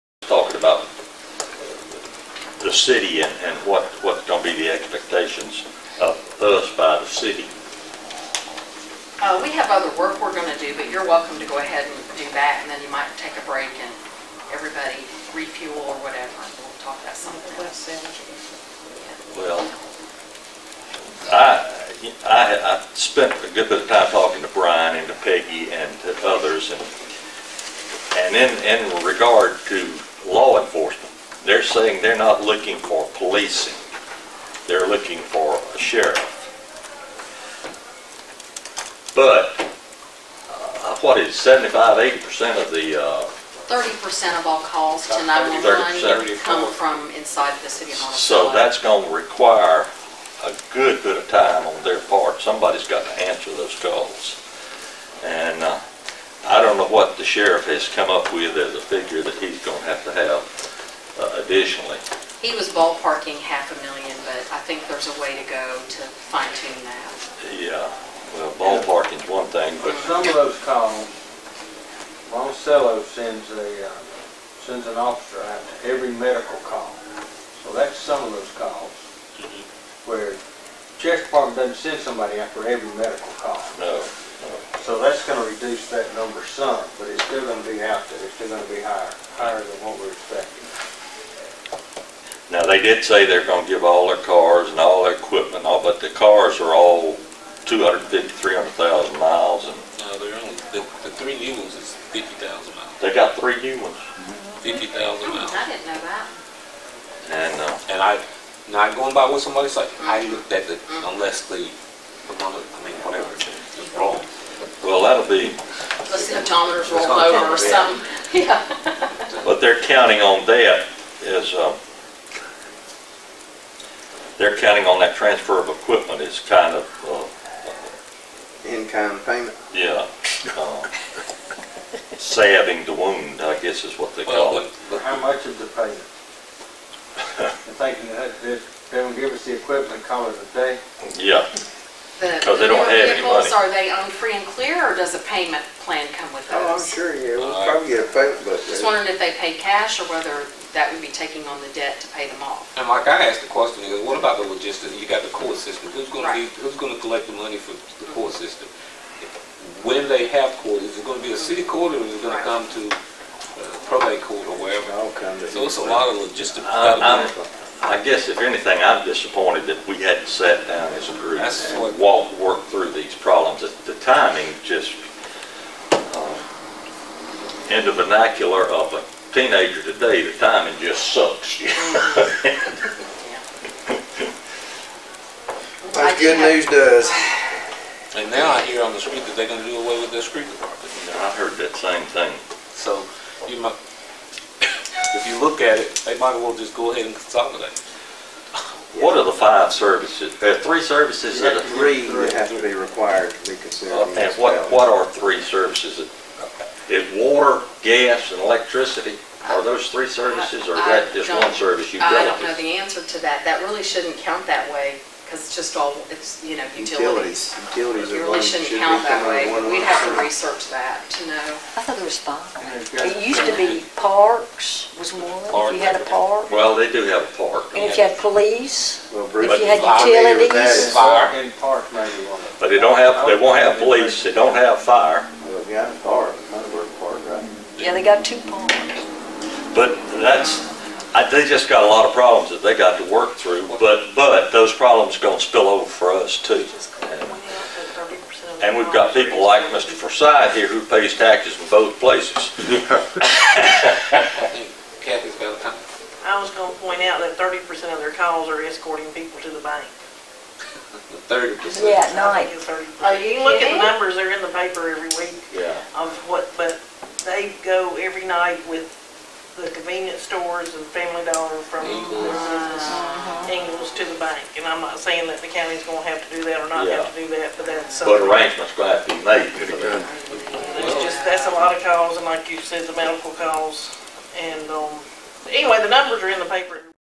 City of Monticello end police services commented on by JCBOC pt I
BOC Budget work session 5/20/14